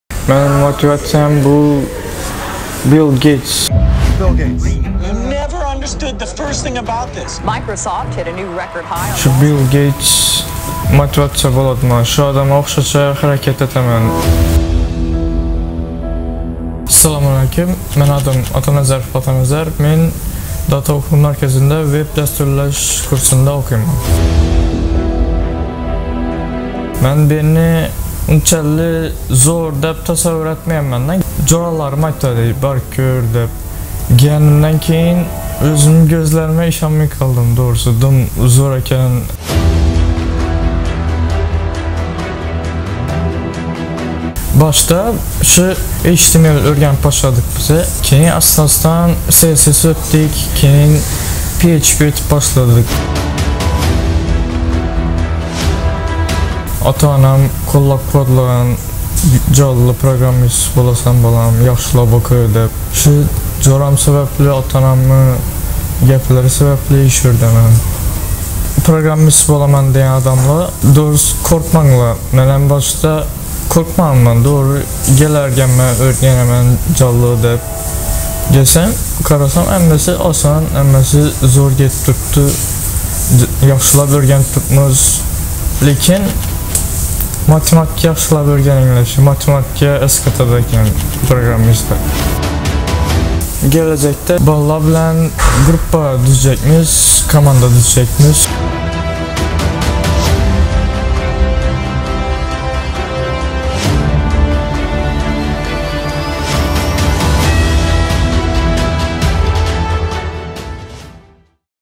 💣Har bir bo'lajak dasturchi uchun TOP maslahatlar!
👨‍🚀"PHP'ga asoslangan web dasturlash" kursi o'quvchisi Otanazar Otanazarov o'qish jarayonidan kelib chiqib o'z tengdoshlariga maslahatlar berdi.

👨‍💻Dasturlash kelajak kasbiligini tushunganlar allaqachon harakatni boshlashdi. Sen ham ular kab bo'l, harakatni boshla!

Kursda o'qishni xohlovchilar hoziroq adminstartorlar bilan bog'laning!